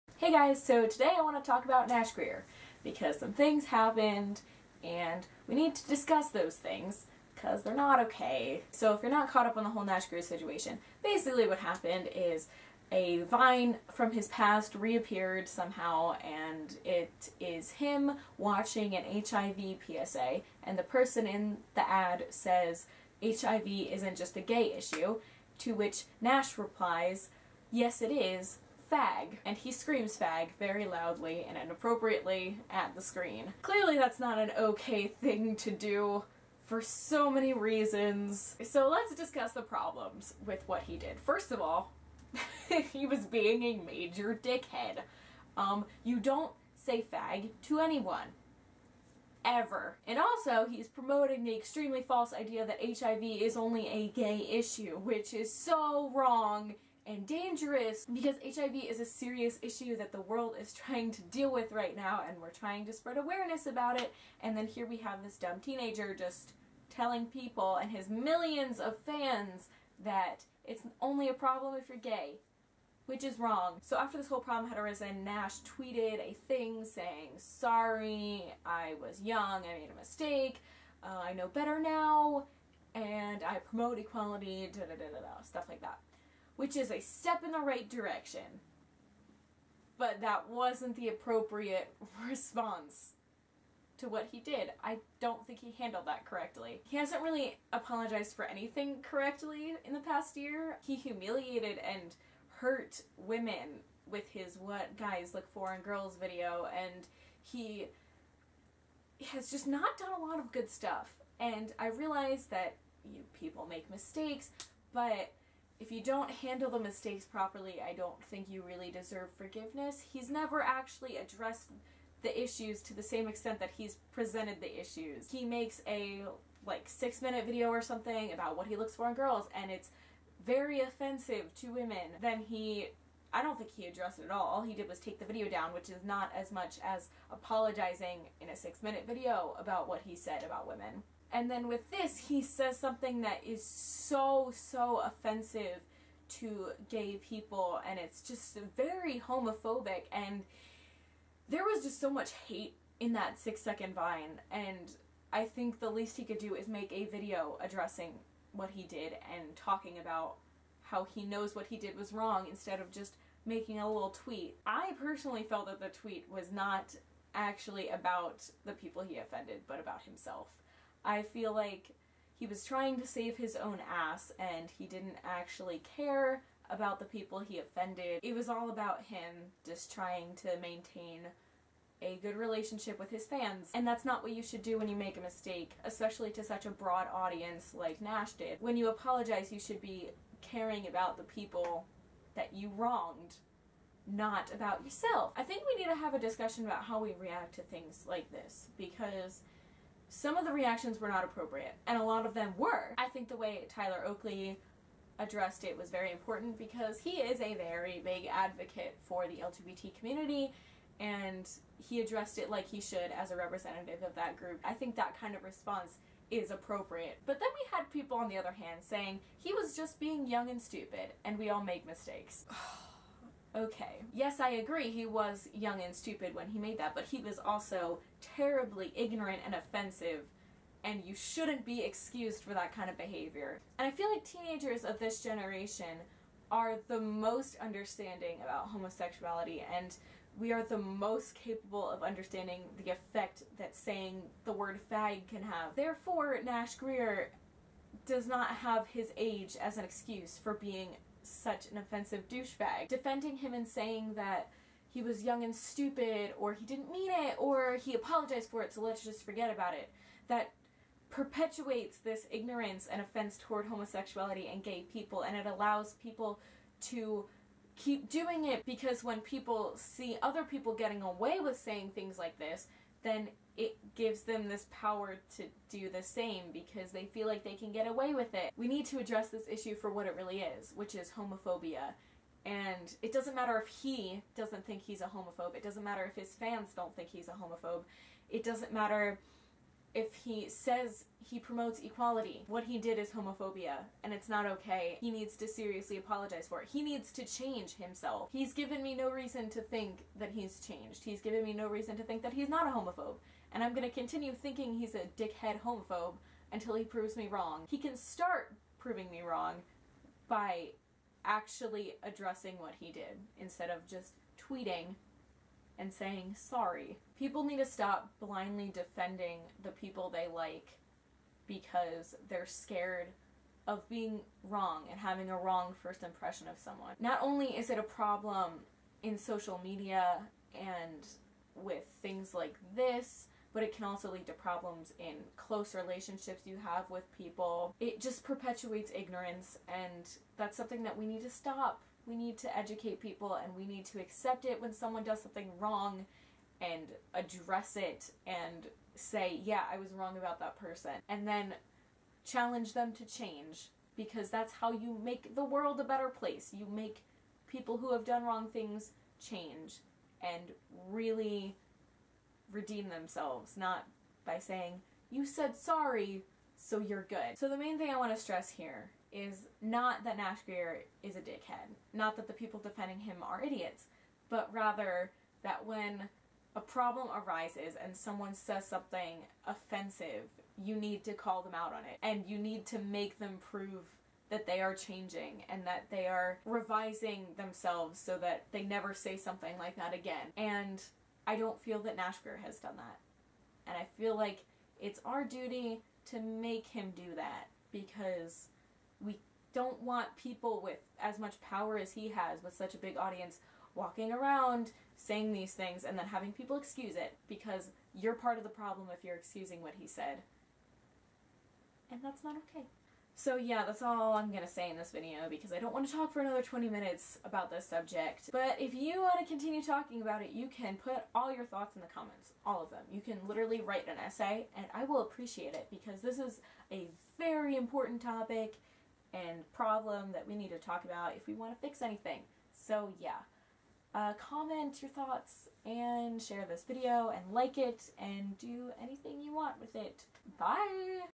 Stop Excusing Ignorance
In which I talk about Nash Grier.

I posted this earlier but I fucked up editing and accidentally made it 25 minutes long, so apologies to those who commented on the first upload. Y'all had some good comments.